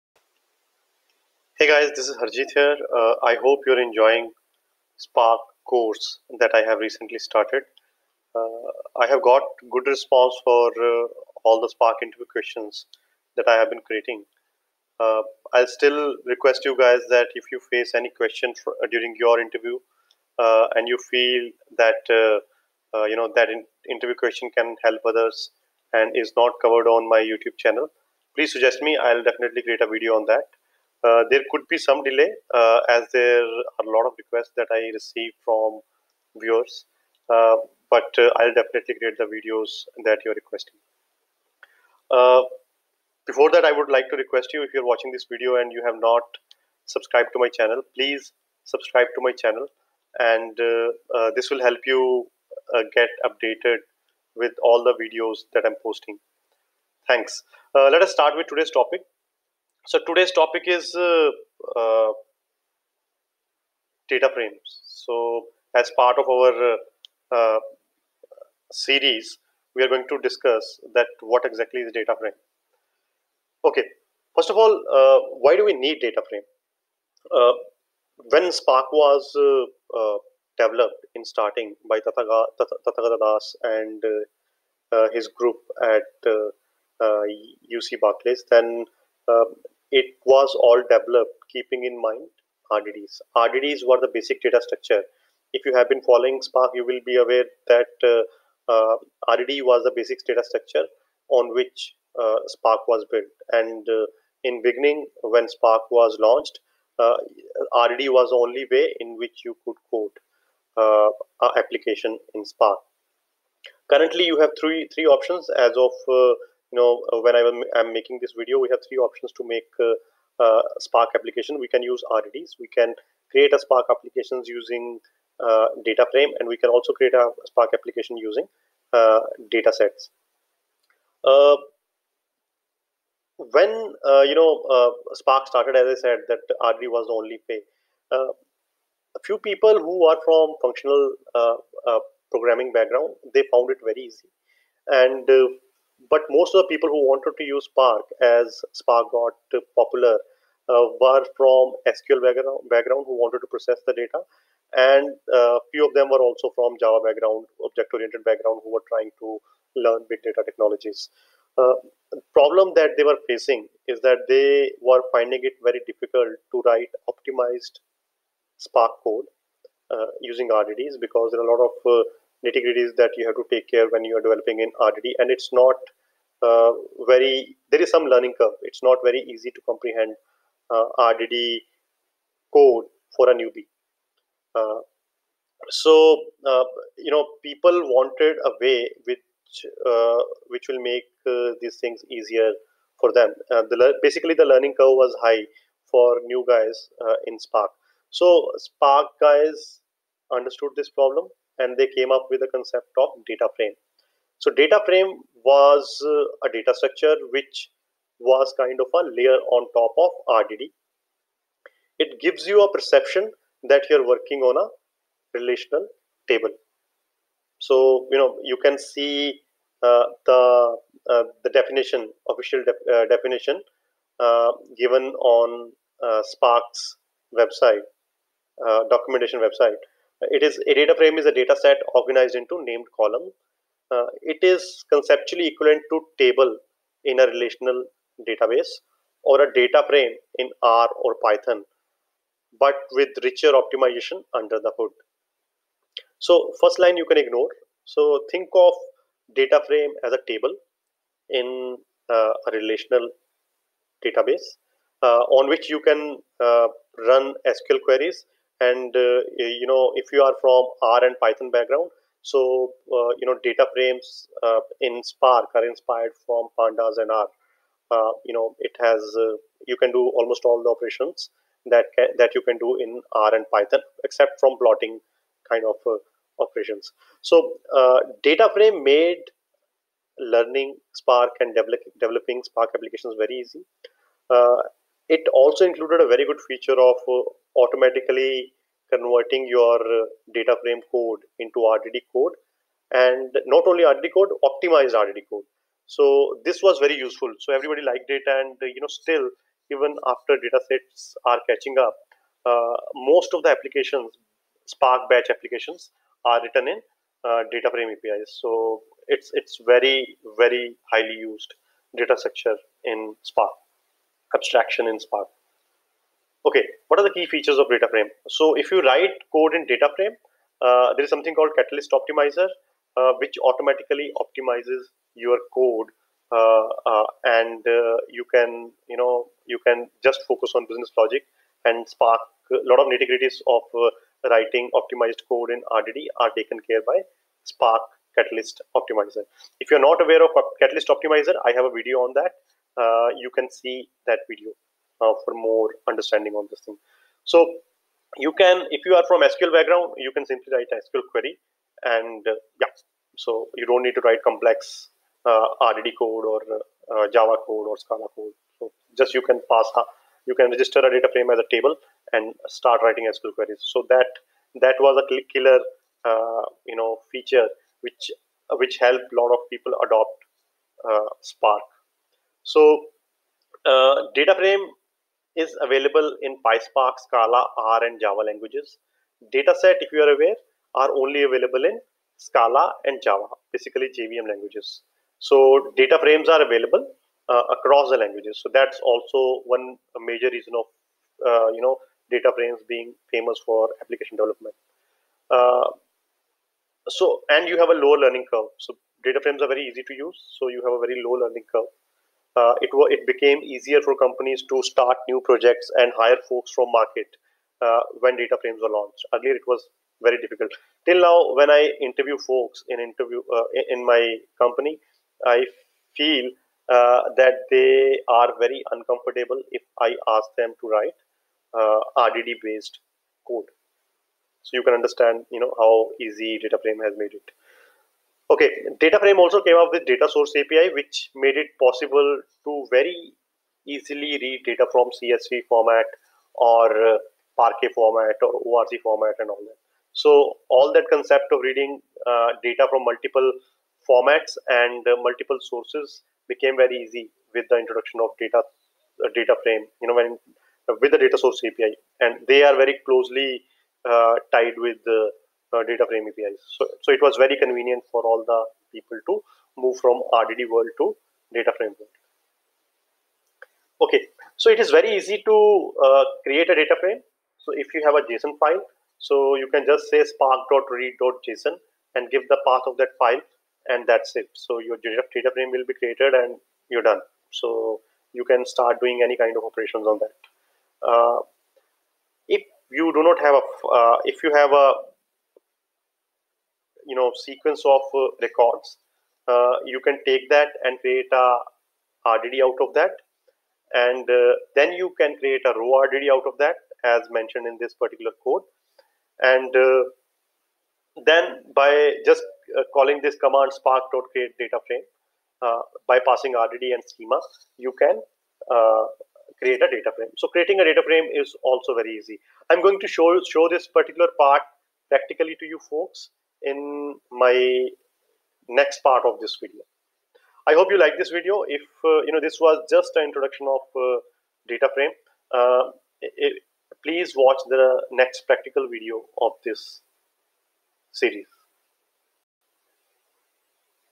4.1.1 Spark DataFrame Introduction | Part 1 | Spark Tutorial
As part of This video we are Introducing spark dataframe. Spark dataframe revolutionalzed spark applications.
Spark dataframe made it very much possible to use spark sql by registring dataframe as spark table.
As part of this video we are
1. Introducing Spark DataFrame
2. why We need spark dataframe
3. How to create spark dataframe from a file
4. How to create spark DataFrame From a RDD
5. Key Features of Spark dataframe

Part 1 of this Video Covers Spark dataframe Theory and philosphy
Part 2 of This video covers Spark dataframe basic practical.